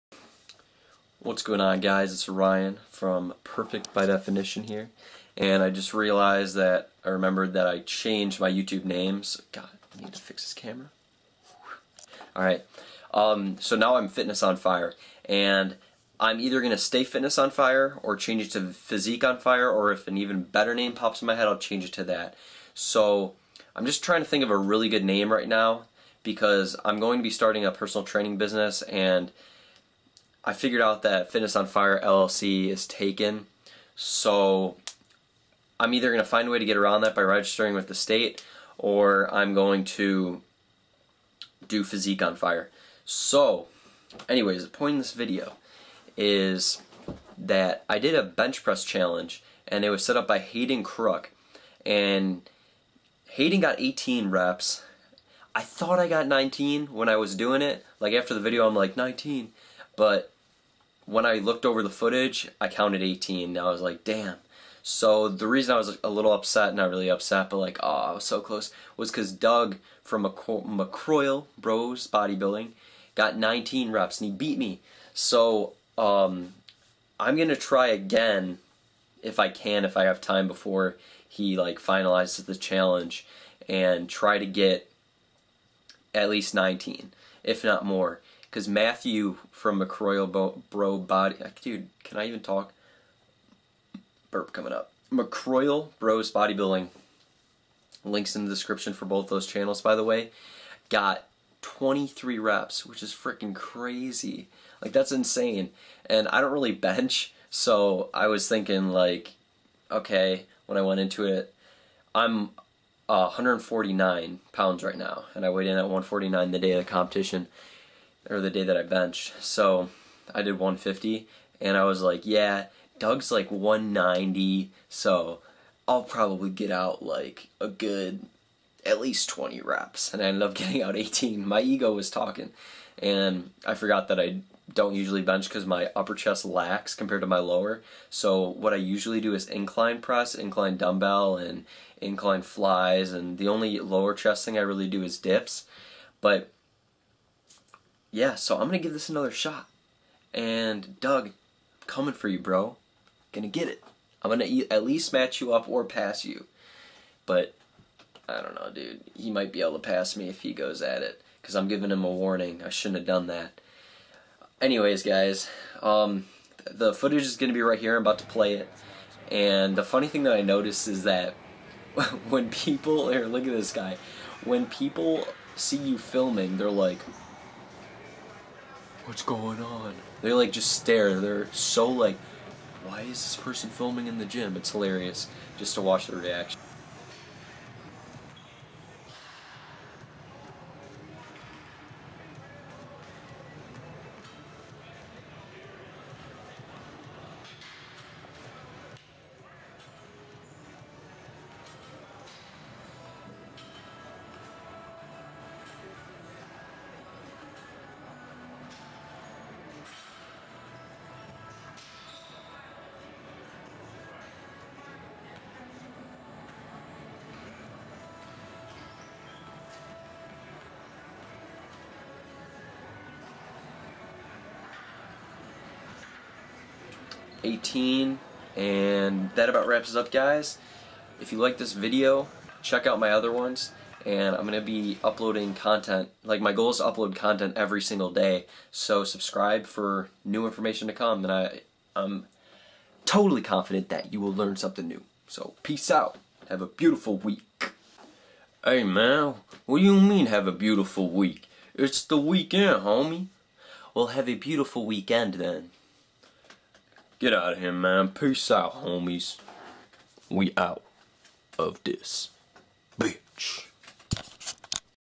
Body Weight Bench Press CHALLENGE!
My first attempt at Haydn Crook's body weight bench press challenge! I got 18 reps out which is one away from Doug's 19 and 5 away from Matthew's 23! I matched Hayden but watch out Doug, i'm coming for you!